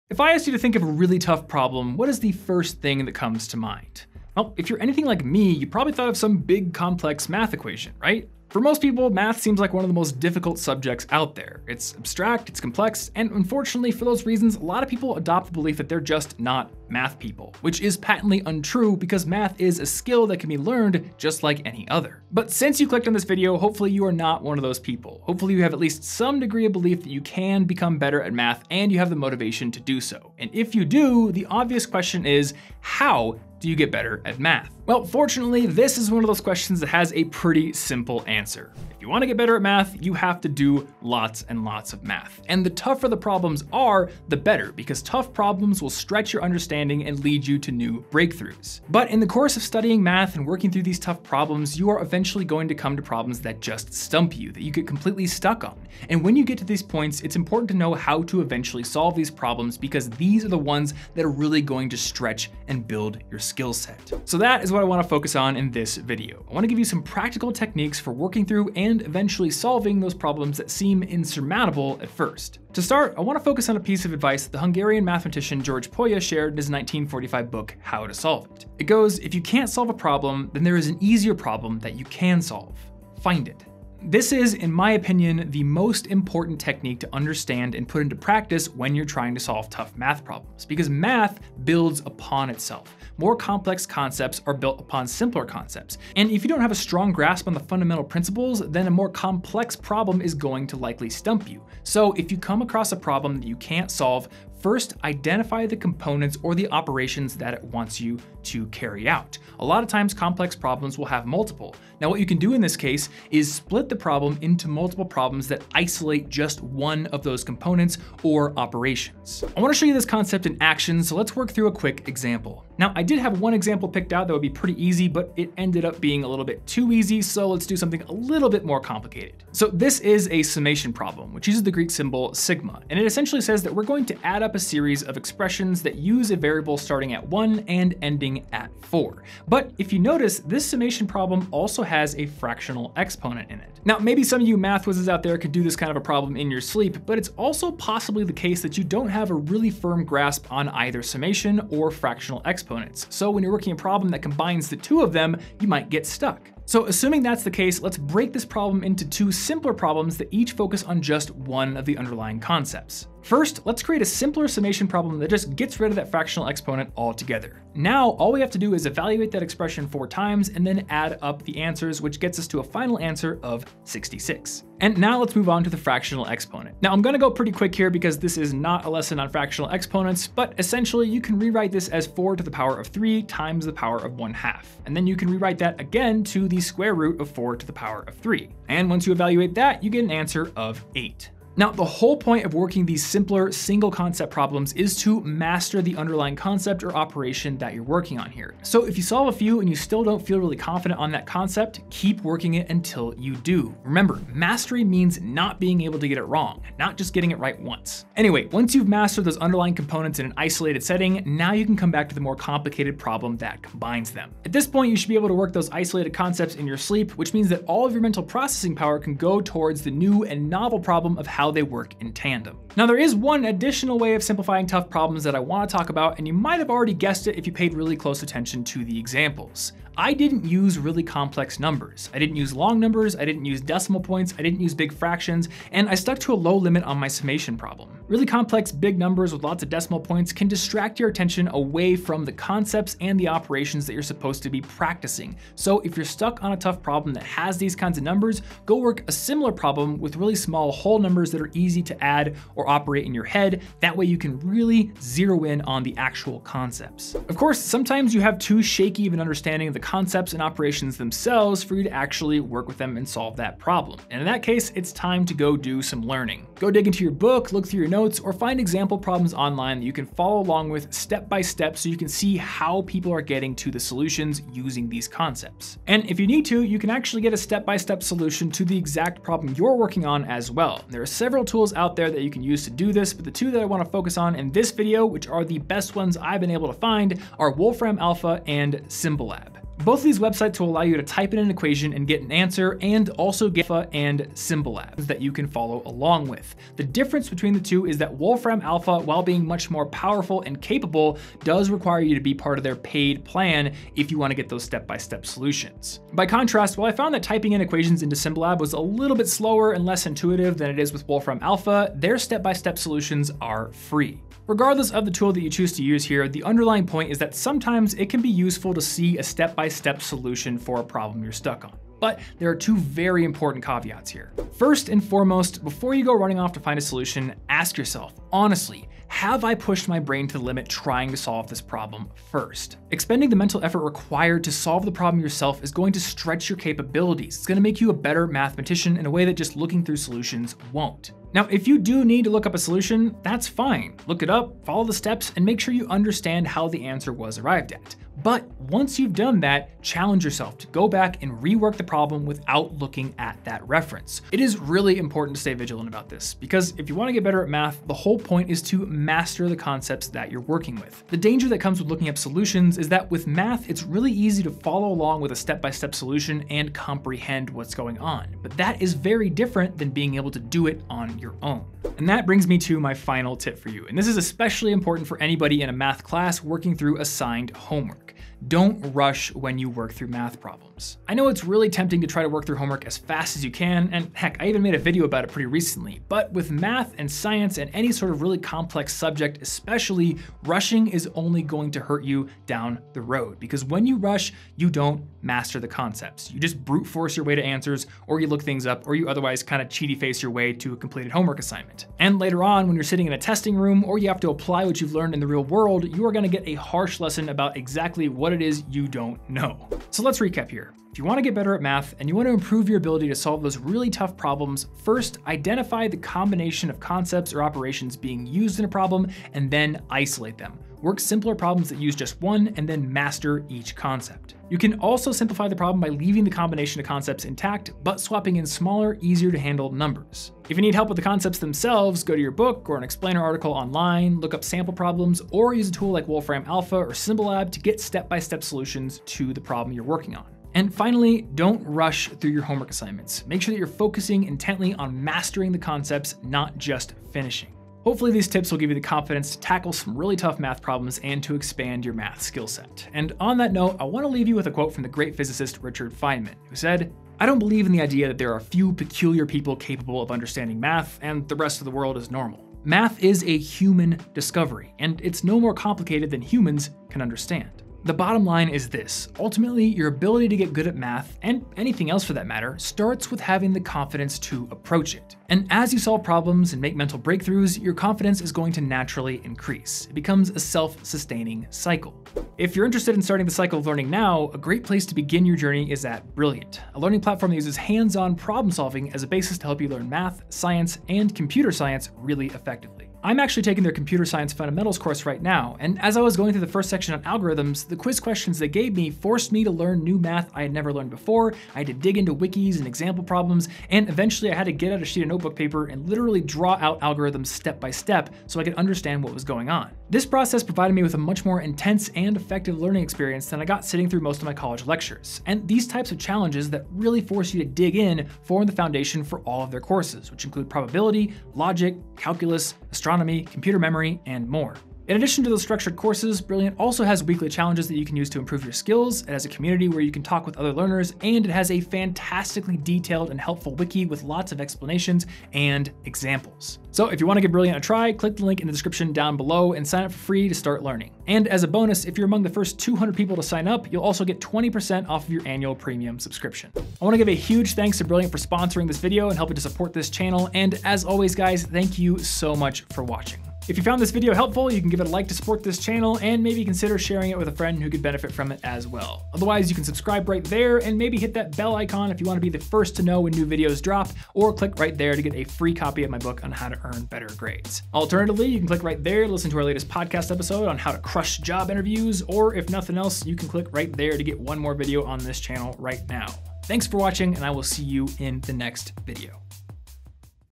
How to Get Better at Math
If you want to improve your math skills, you need to do lots of math. But how do you progress when you come across a problem that seems impossible? Well, if summoning the spirit of Jeff Goldblum doesn't work, the tips in this video just might.

My book "10 Steps to Earning Awesome Grades" is completely free, so check it out if you're interested in improving your grades!

Extension for better control over playback speed (if I'm talking too fast) ➔

~ created by Thomas Frank